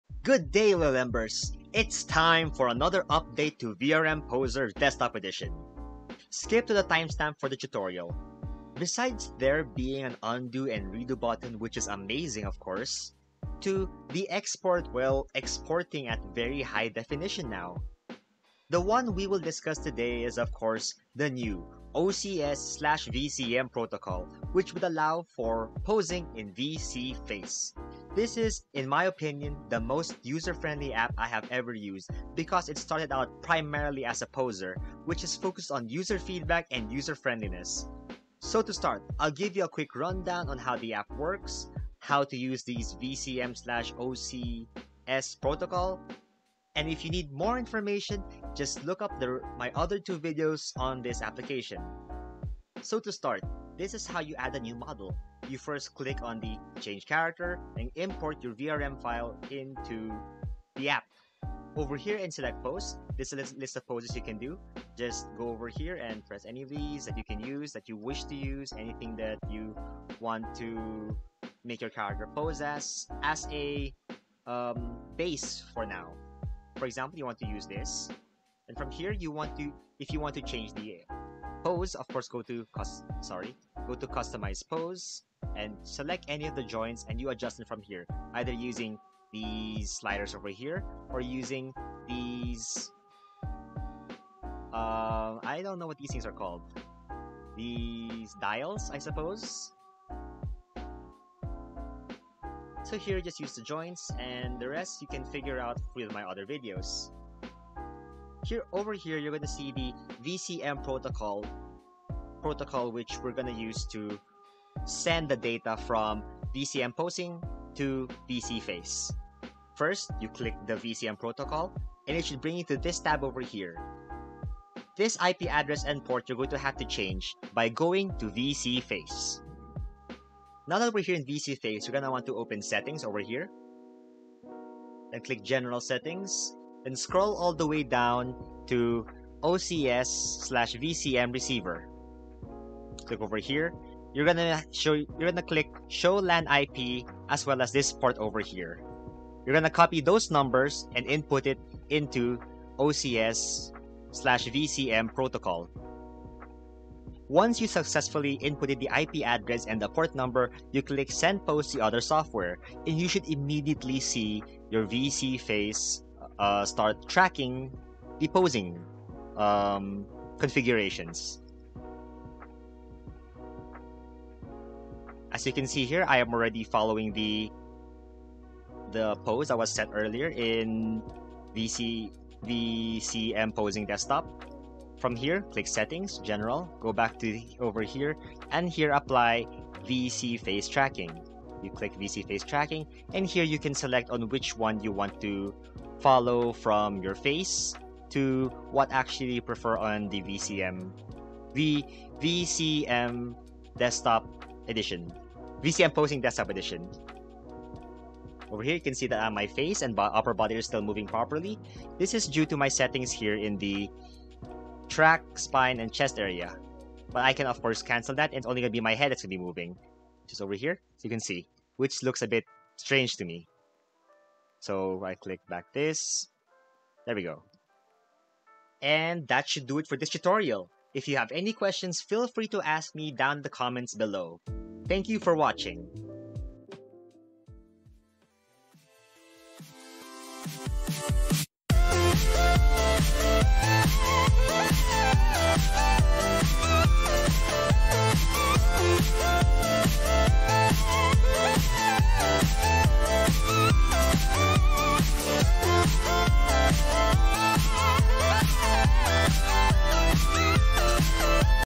NEW Vseeface posing vtuber addon using VRM posing
the new update for Vrm posing allows for ocs/vcm tracking  in vseeface with a bit of effort you can use this app seamlessly

NEW posing in Vseeface using VRM posing
🔥You'll find me here on these sites 🔥